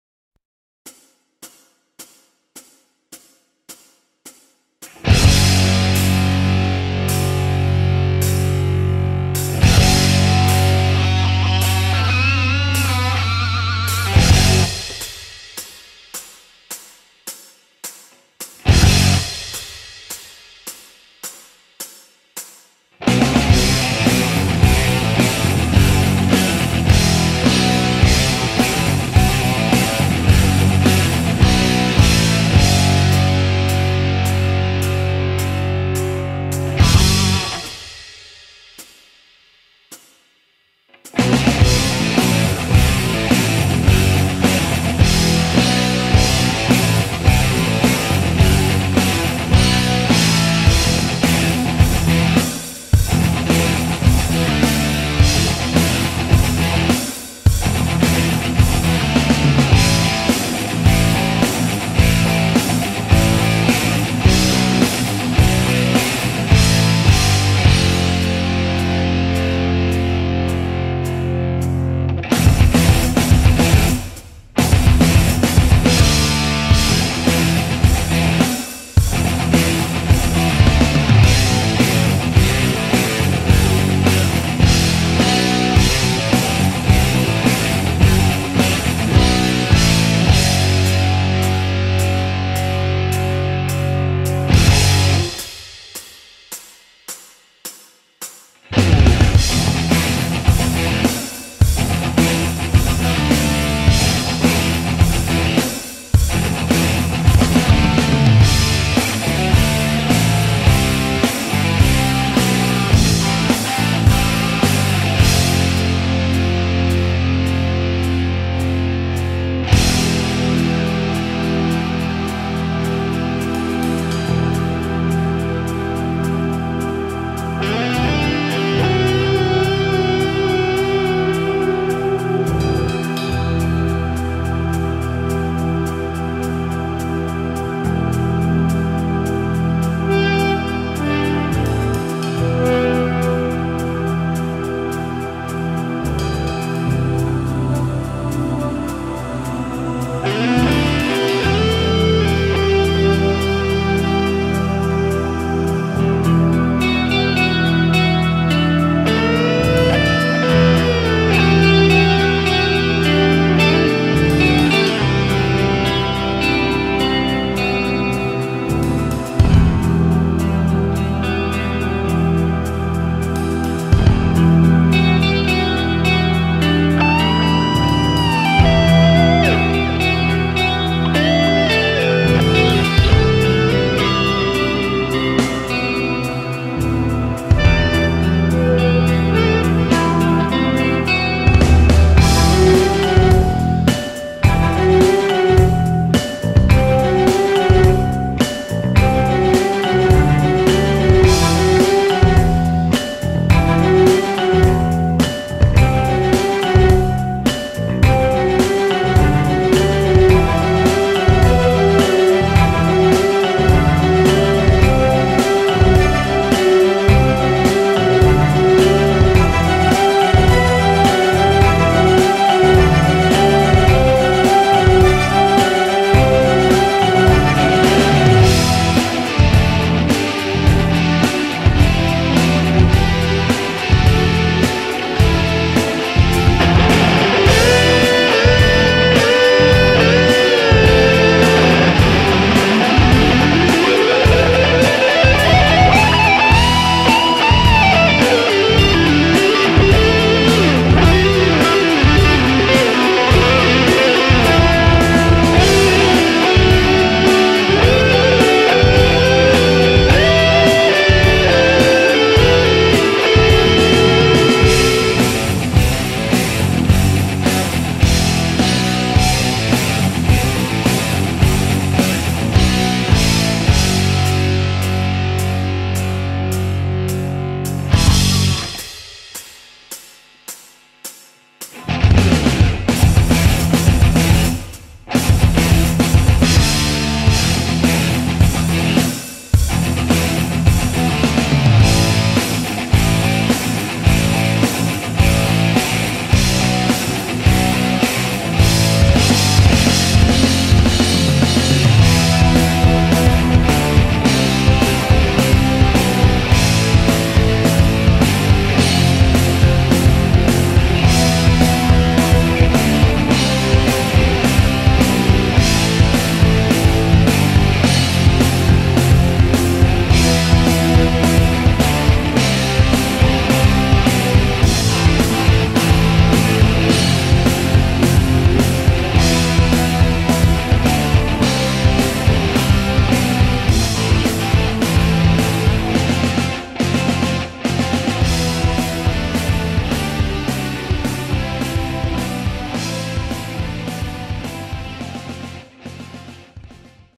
Construção de guitarra(Building an Electric guitar) part-1
Corpo em mogno, braço em maple, escala em ébano, floyd rose special, Tarrachas Gotoh, trastes em aço inox Jescar, elétrica Gotoh e captadores Malagoli custom PTR.